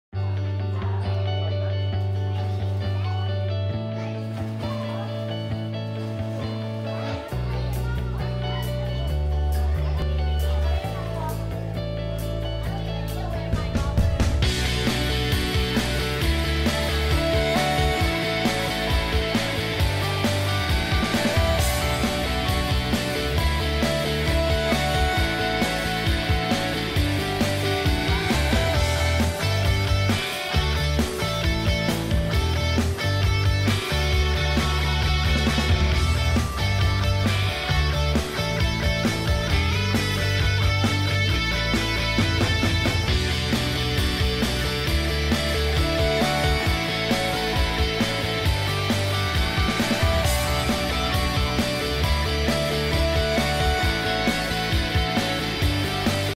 Diving in swimming - peer coaching
Students coach each other in order to try and improve their partners’ technique. They use kick boards to stretch and dive out over before going into the streamline position.